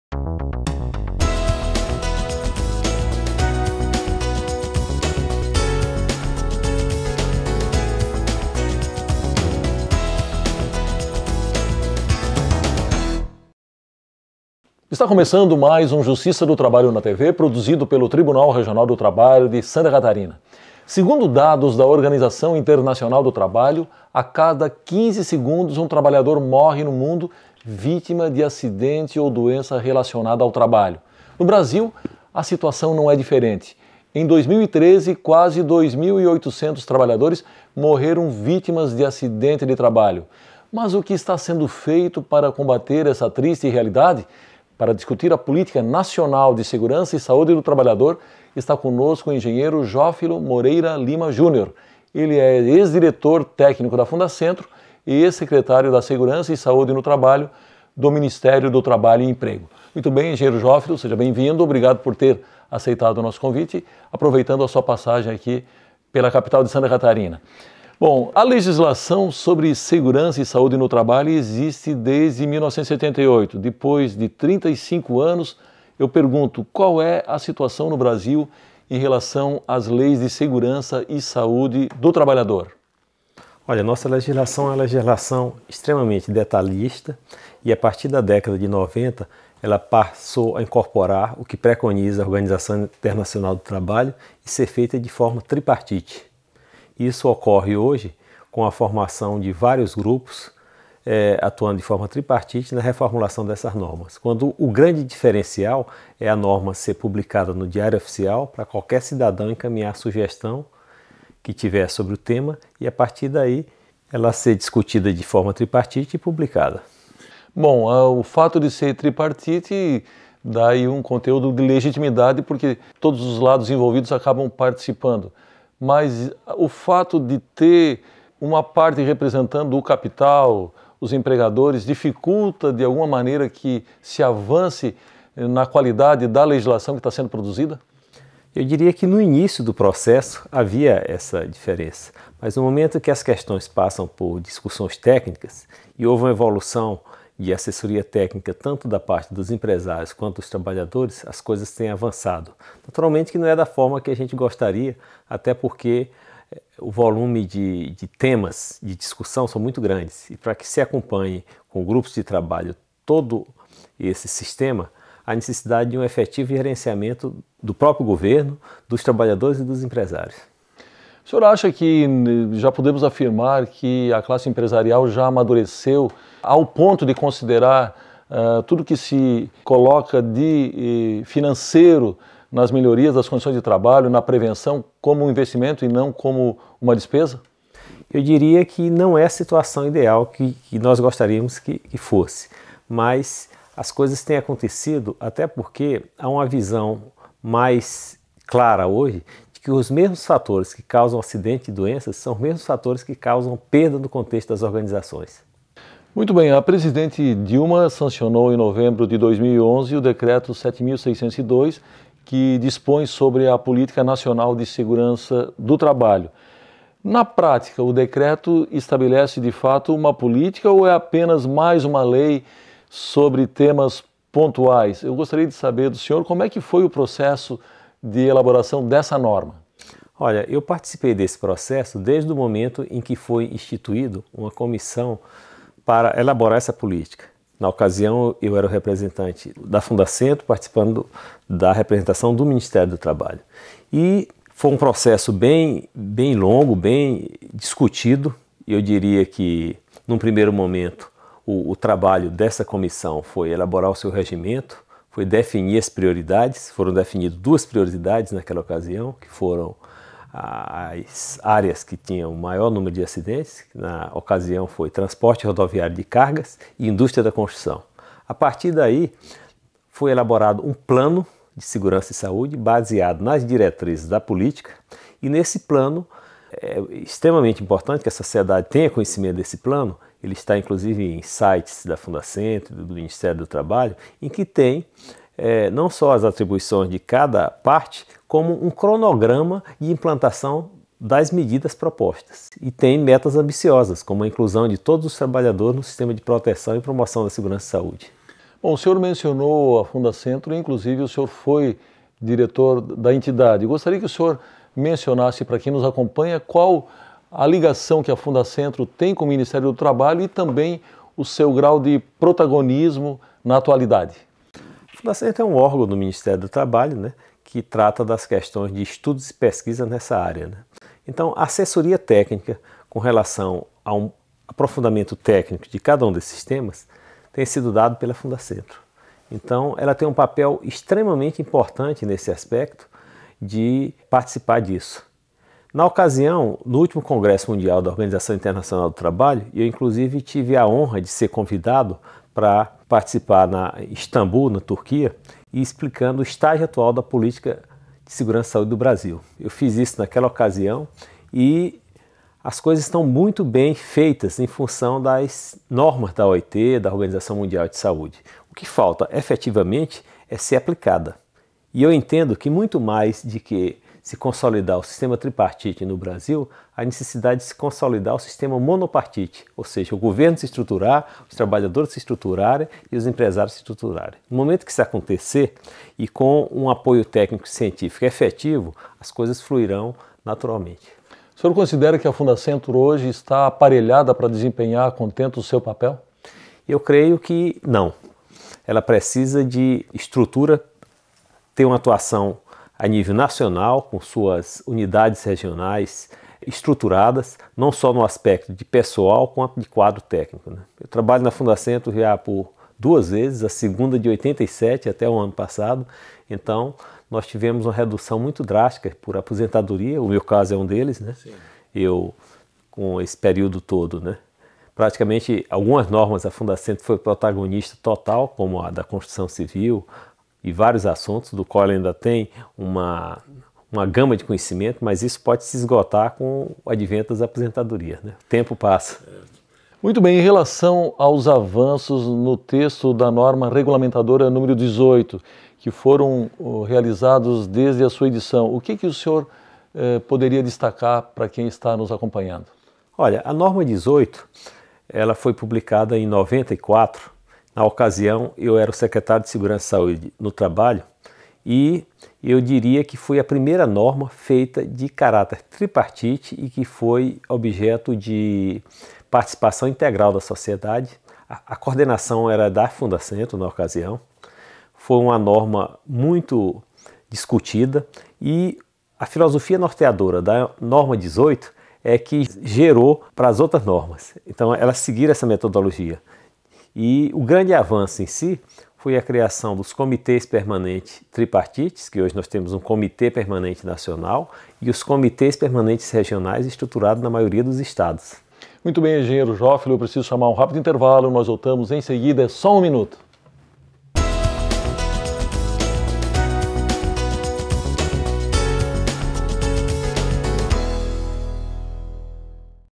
Bloco 1/3 - Segurança e saúde no trabalho
O engenheiro civil Jófilo Moreira Lima Júnior é o nosso entrevistado. Ele discute a política nacional de segurança e saúde do trabalhador. Moreira foi titular da Secretaria de Segurança e Saúde no Trabalho, do Ministério do Trabalho e Emprego (MTE), e diretor técnico da Fundacentro.